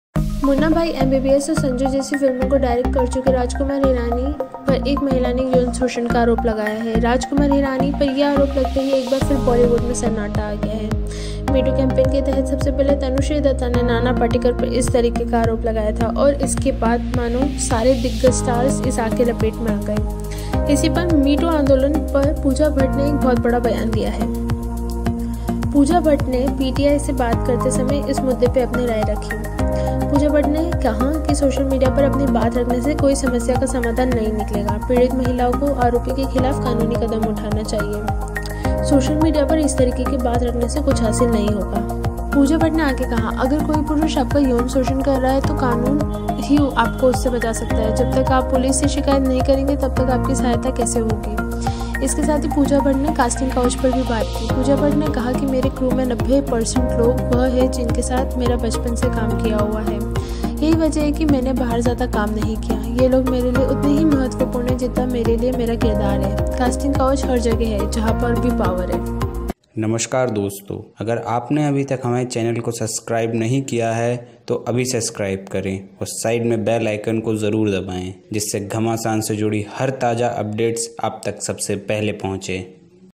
MeToo Pooja Bhatt's | Big Disclosure, CASTING COUCH everywhere in INDUSTRY
ACTOR and FILMMAKER Mahesh Bhatt Daughter Pooja Bhatt | BIG STATEMENT on the MeToo Movement, which gives a BOLD STATEMENT on every issue in Bollywood Industry. Pooja Bhatt on MeToo, Pooja Bhatt on threatening Girls.